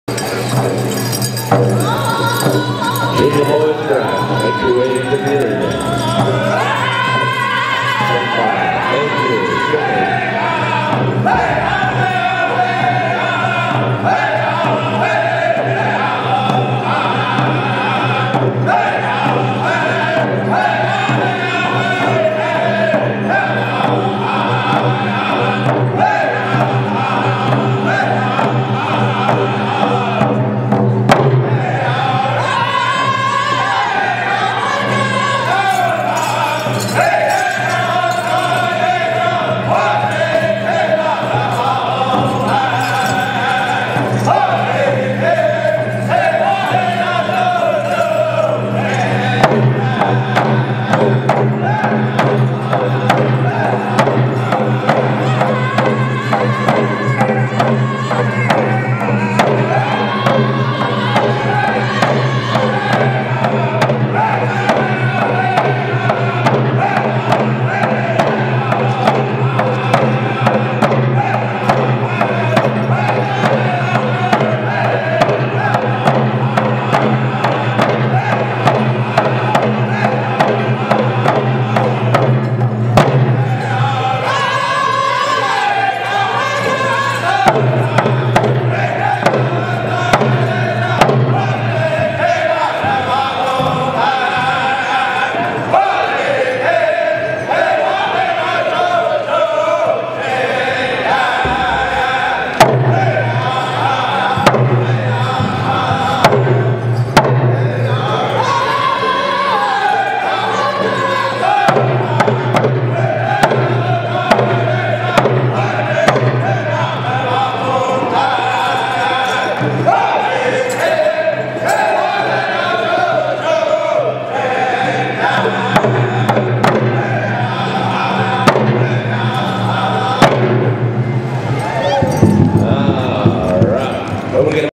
Porcupine Singers
Kerwin E. Condon Hand drum contest (1st round) @ He Sapa Wacipi 2009